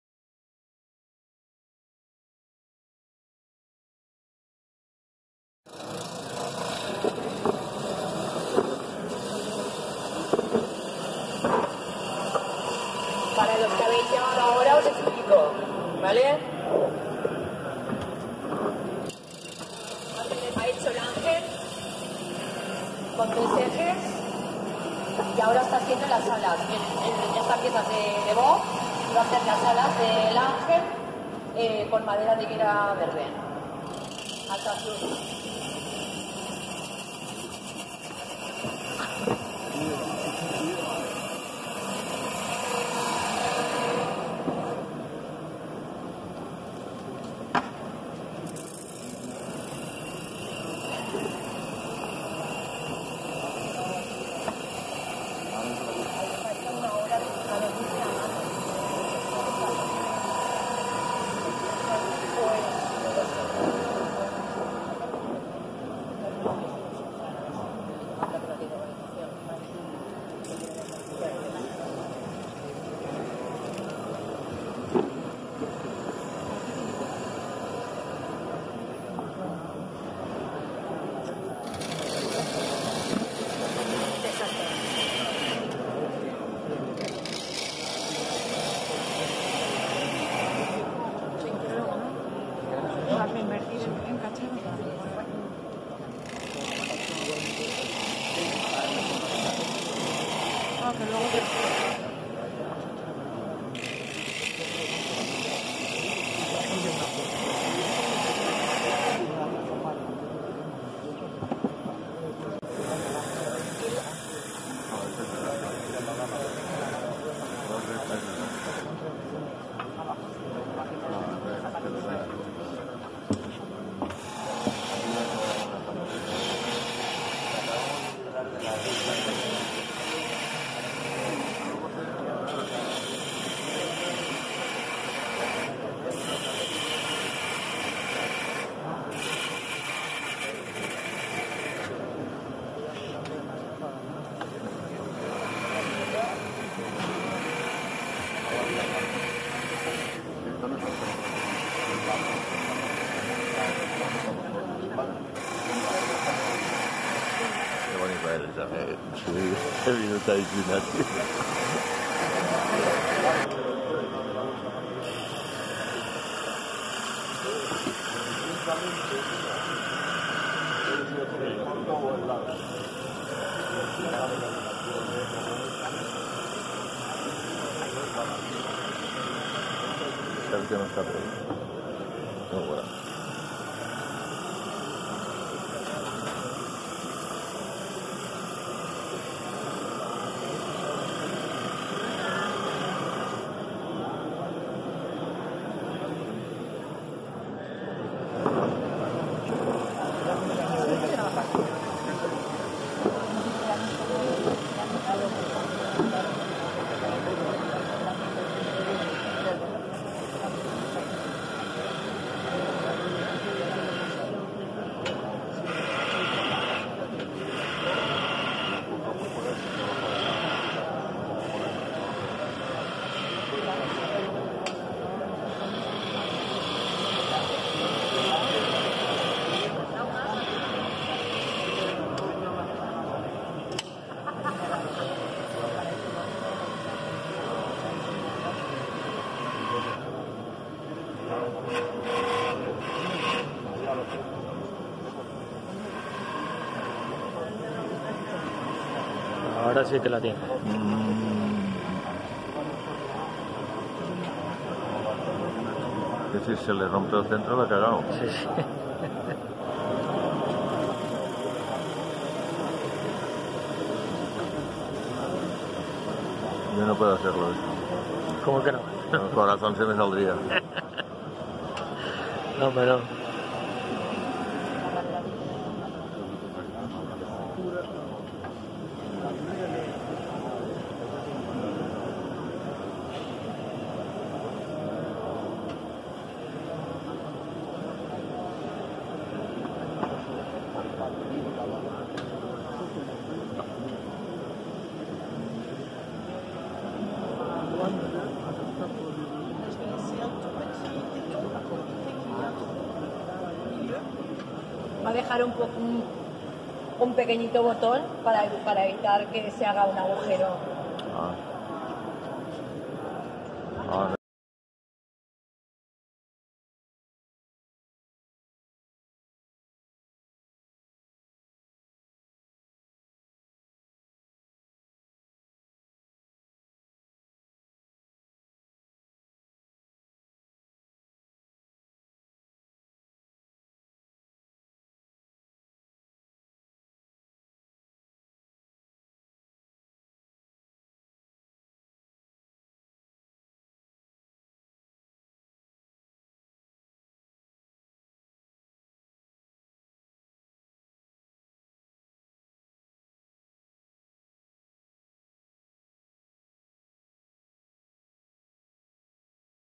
X Jornadas de trabajo en torno Vilafamés 2020. JOSS NAIGEON.
Ángel guardián realizado por Joss Naingeon con higuera verde.